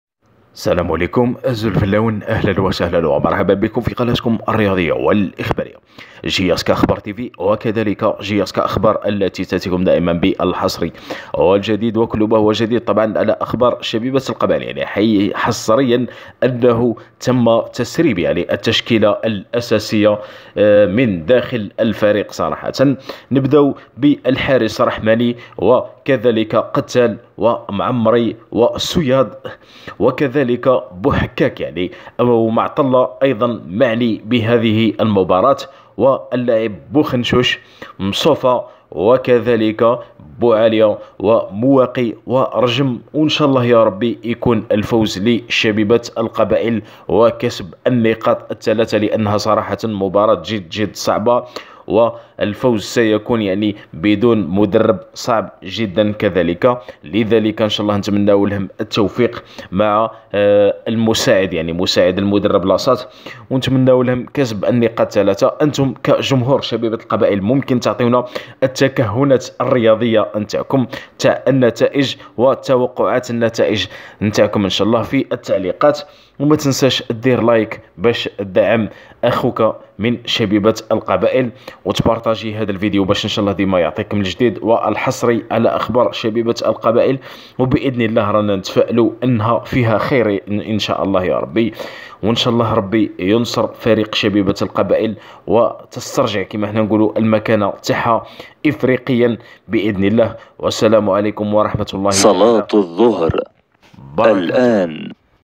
تم تسريب التشكيلة الاساسية لفريق شبيبة القبائل المعنية بمباراة اليوم امام اتحاد العاصمة jsk
التشكيلة الاساسية لشبيبة القبائل لمباراة اليوم امام اتحاد العاصمة